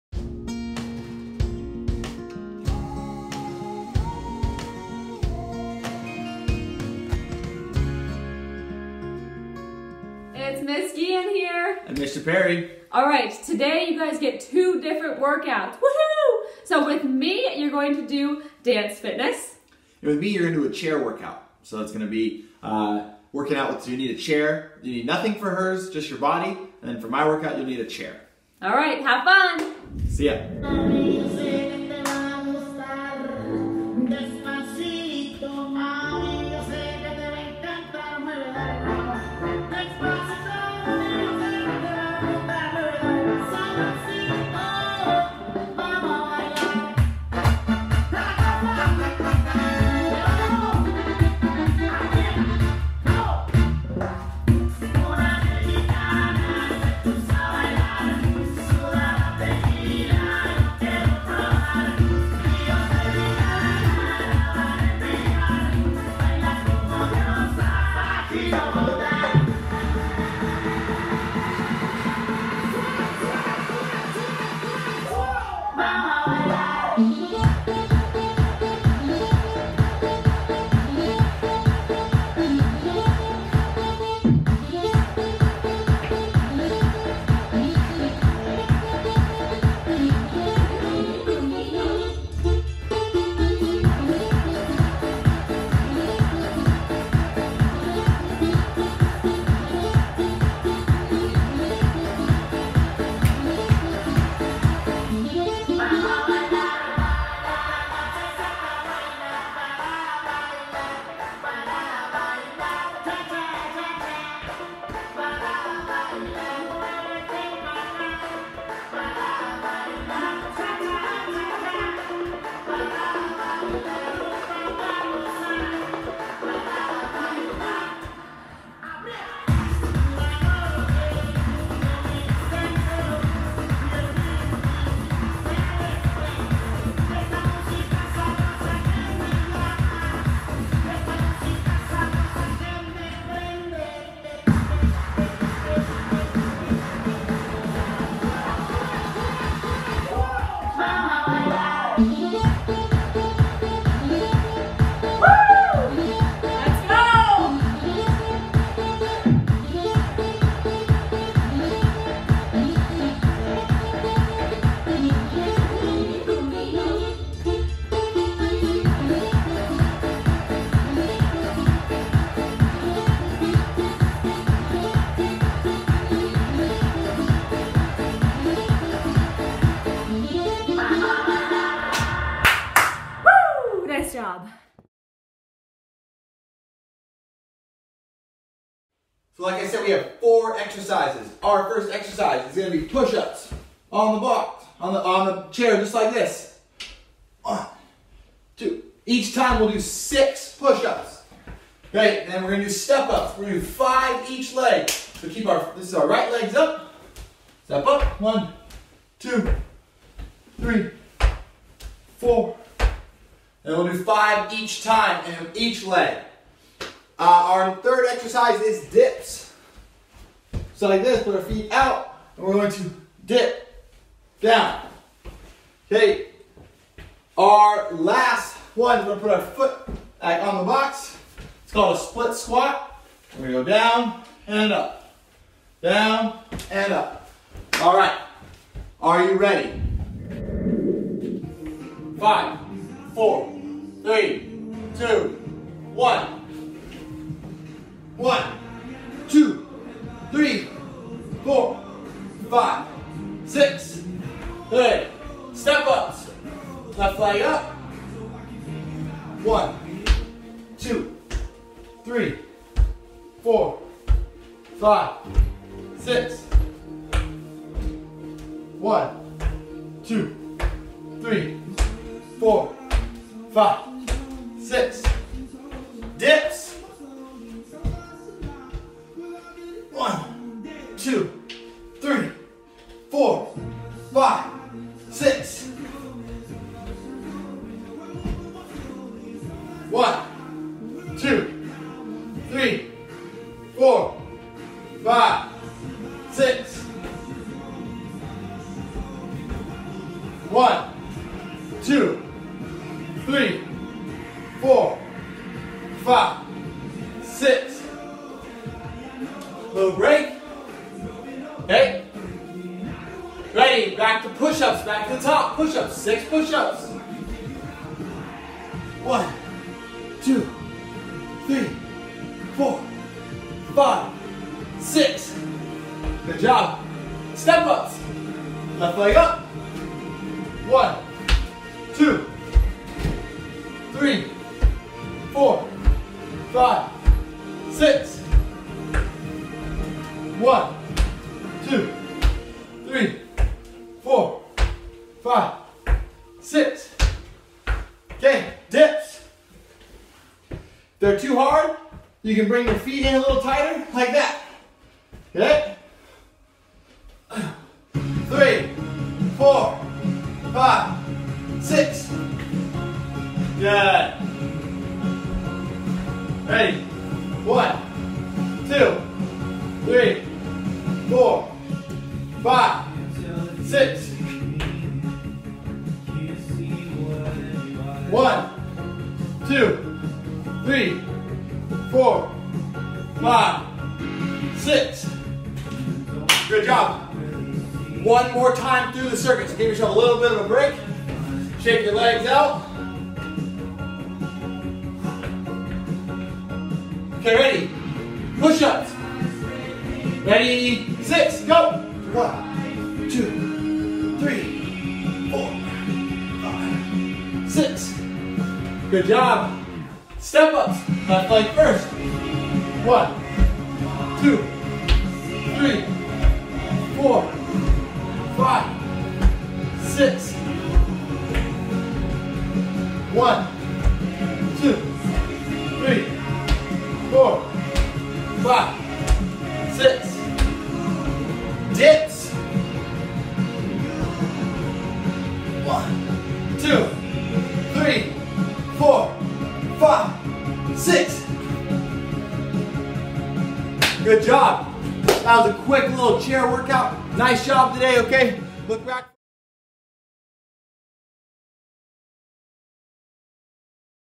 Dance Fitness and Chair Workout
Double Workout video! Dance Fitness with MS. Guillen and Chair Workout with Mr. Perry!